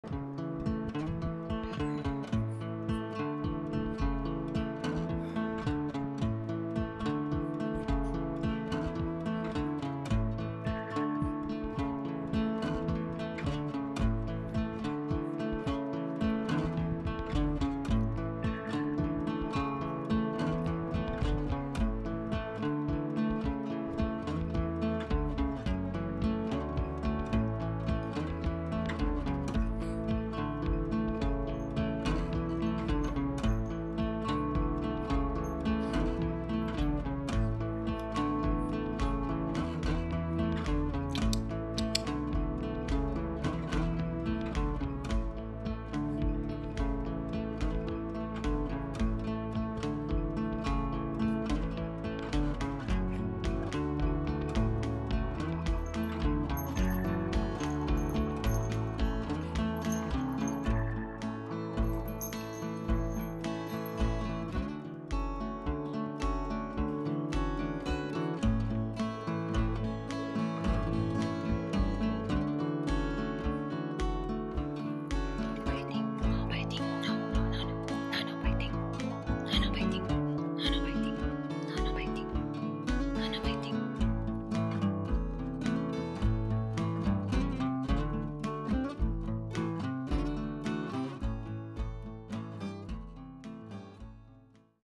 Beagle Puppy Reaction to her New Dress - Cute Beagle Puppy Reaction Video
Beegi Vlog 88 - Beagle Puppy Reaction to her New Dress - Cute Beagle Puppy Reaction Video